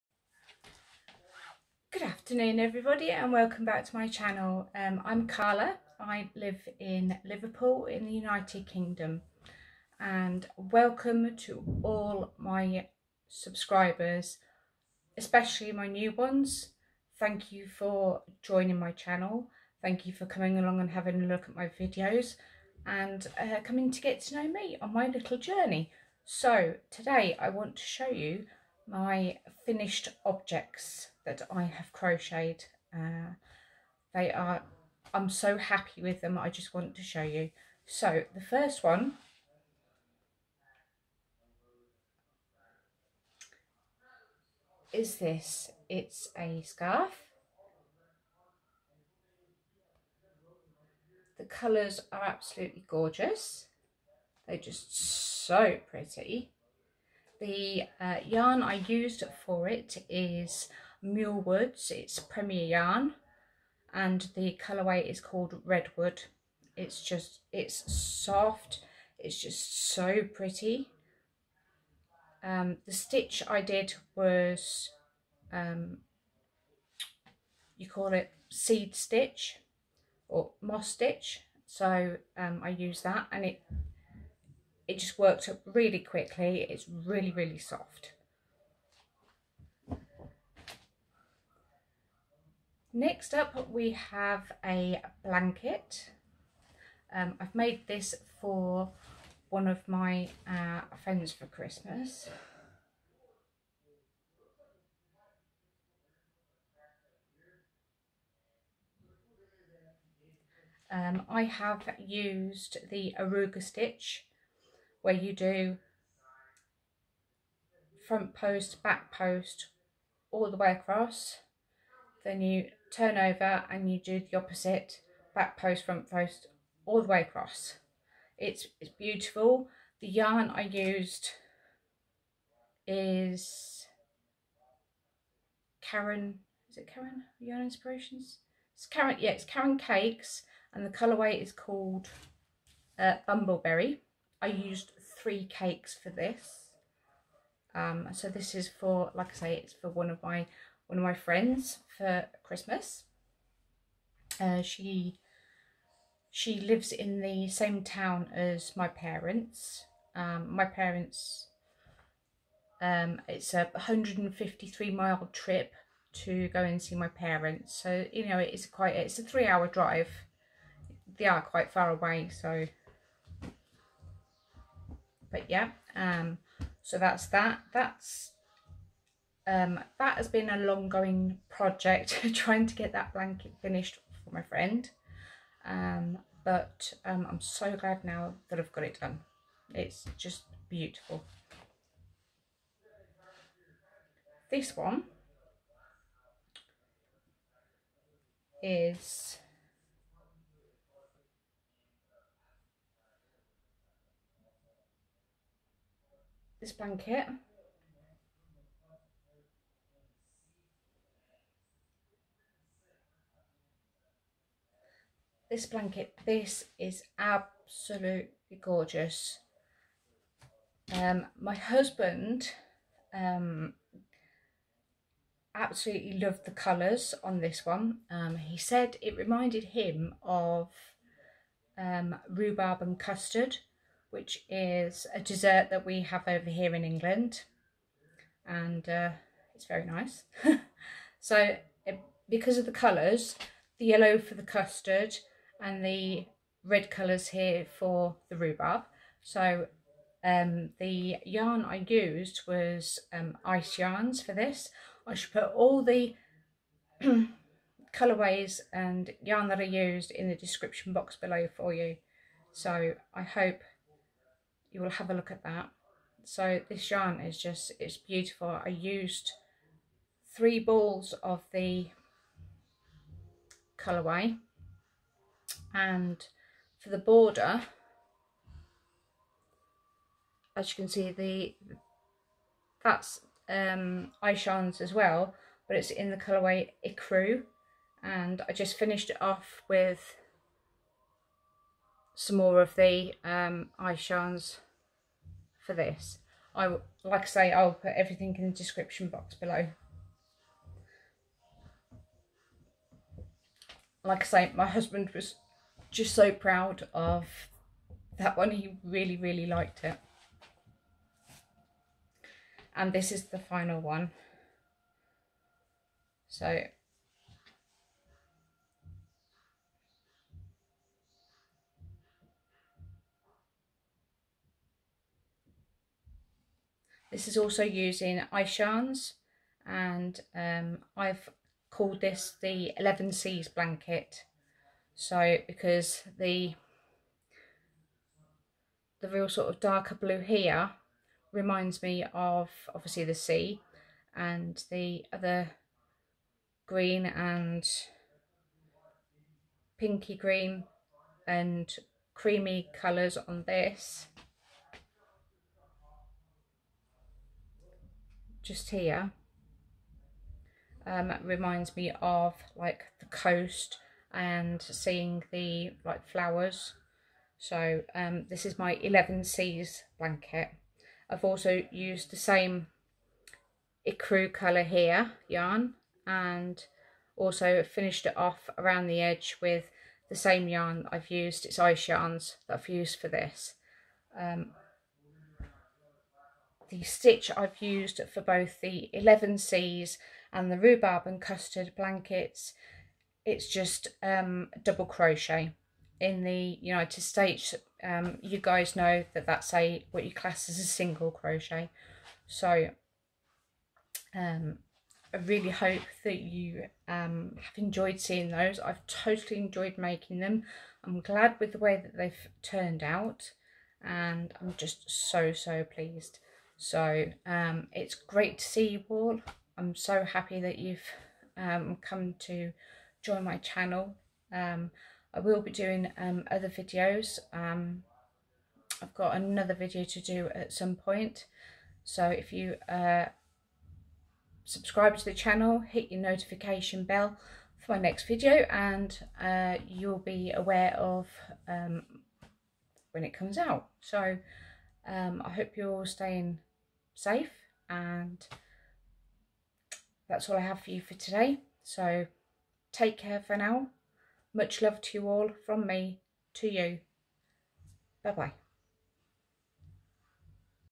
Crochet Finished Scarf and Blankets
I would like to welcome all of my new subscribers and to everyone who's been following my little journey.  I really appreciate you allfor taking time out of your day.

A big thank you to Allison Russell for showing me kindness, friendship and support ❤

The scarf is was with Premier Muir Woods, colourway Redwood.  Lot number 9953

Rhubarb and Custard Blanket is made with Ice Yarns Baby Wool Design in Yellow and white.

The border colour is Ice Yarns Baby Comfort in the colourway Ecru.

The Eleven Seas Blanket is made with Ice Yarns Baby Wool Design in Blue and White.  Number 58388.

Please stay safe and take care 🙂

Kind regards

Carla xx